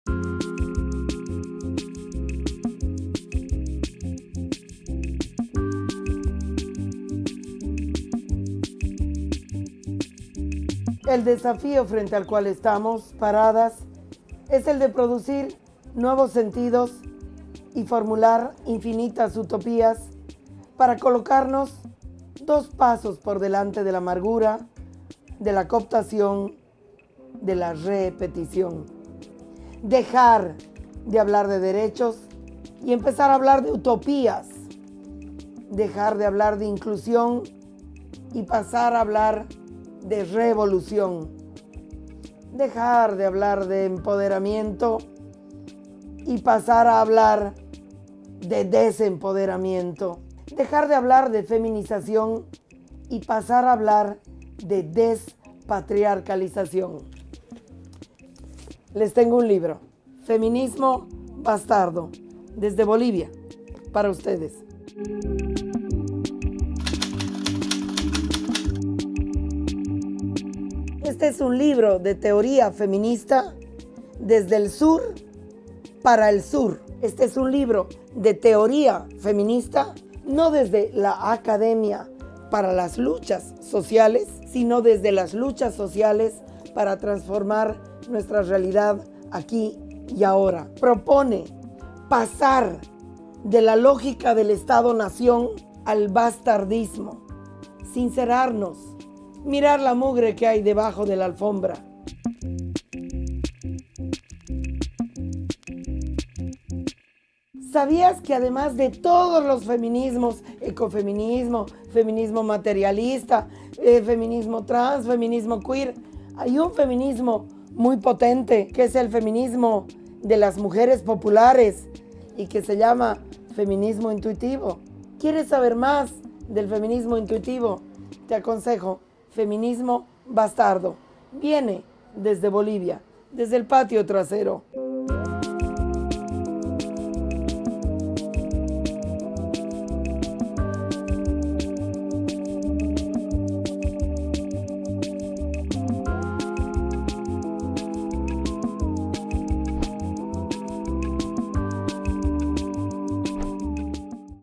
Te tengo un libro cap. 2: "Feminismo bastardo" de María Galindo
Revisa nuestra segunda entrega de "Te tengo un libro". Esta vez, junto a la escritora y activista boliviana, María Galindo.